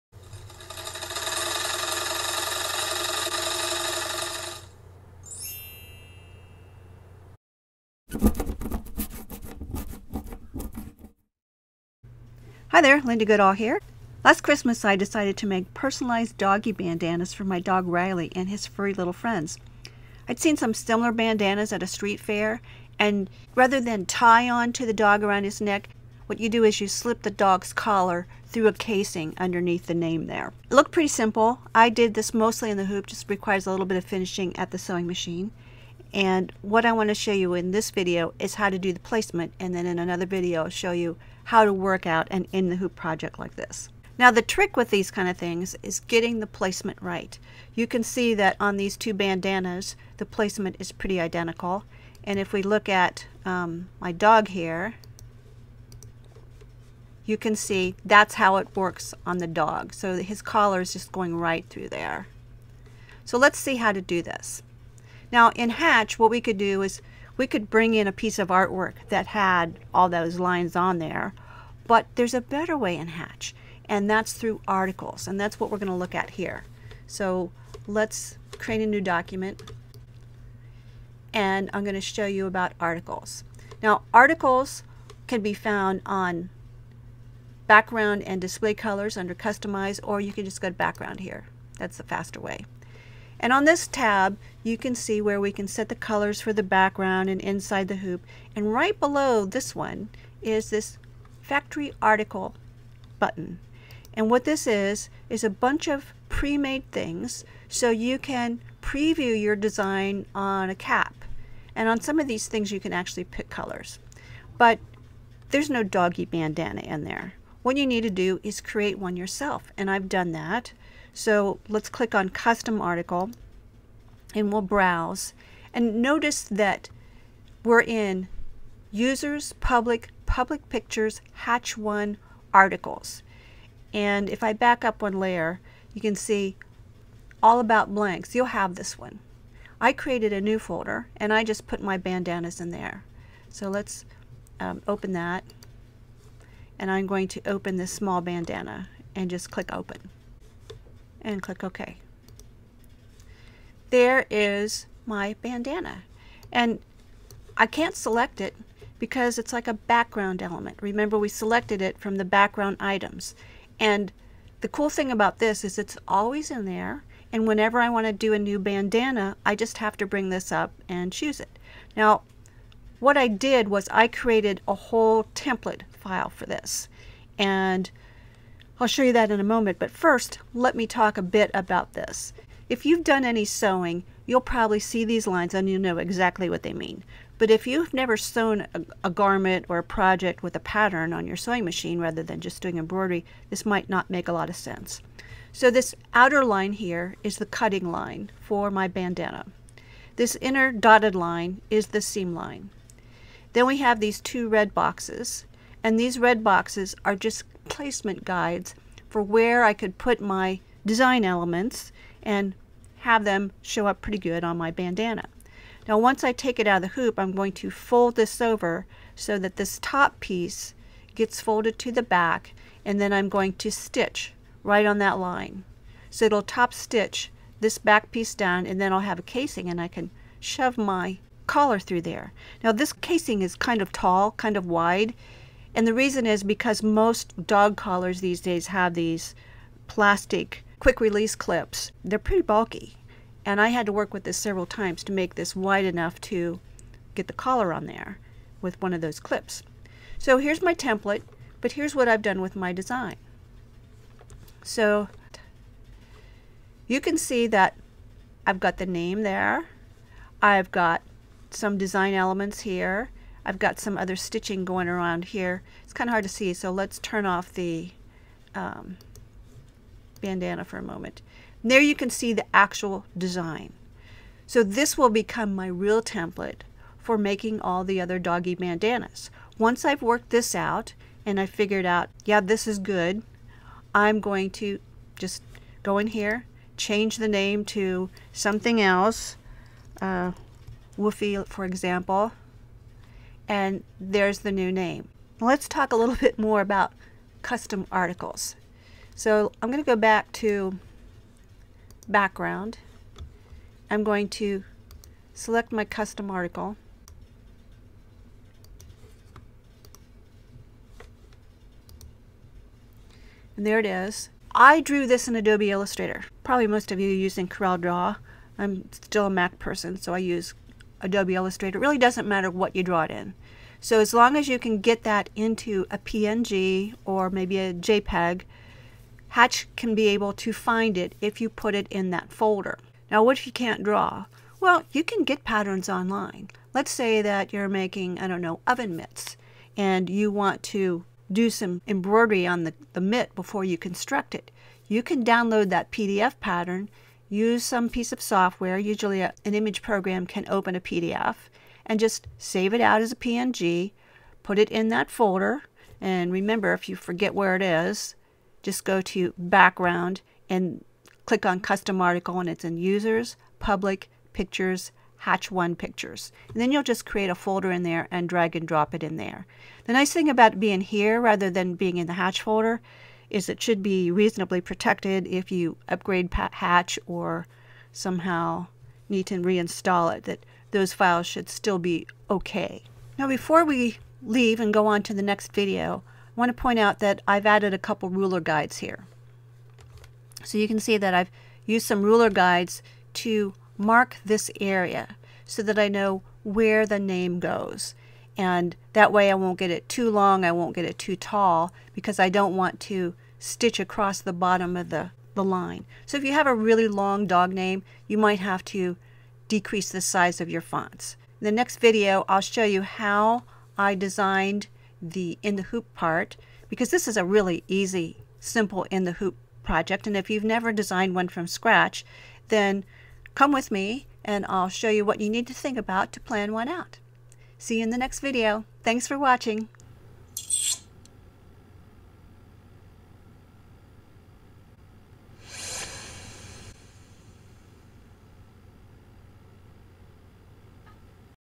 Wilcom Hatch - Using Custom Articles for Placement
Making and loading your own custom placement templates, which Hatch calls articles, is easy and very useful! I'll show you how in this video.